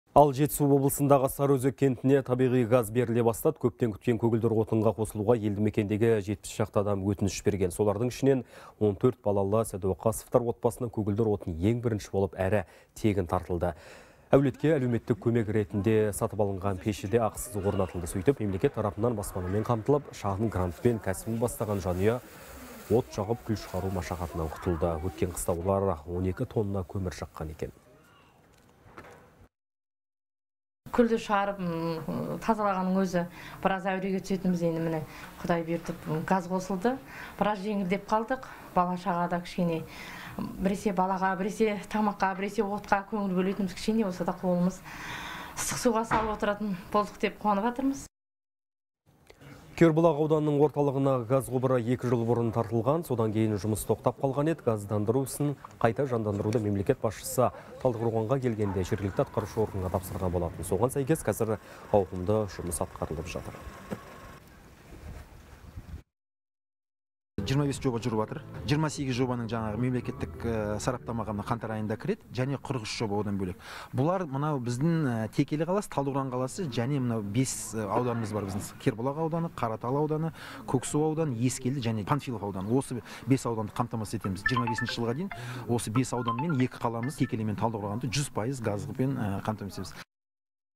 Жетісу облысының Сарыөзек кентіне табиғи газ беріле бастады
Көптен күткен көгілдір отынға қосылуға елдімекендегі 70 шақты адам өтініш берген.